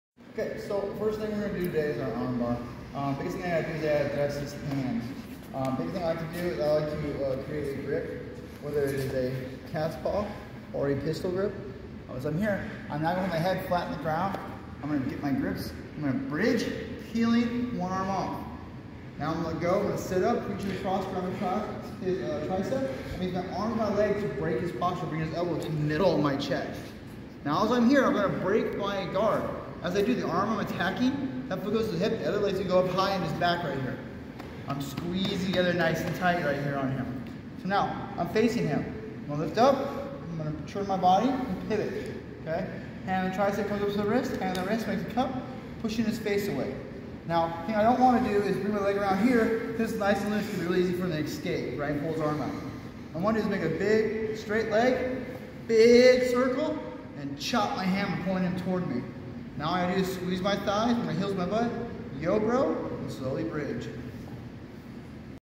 Armbar
In this video segment, we're focusing on the setup of an armbar in your opponents closed guard and how to finish it, enjoy!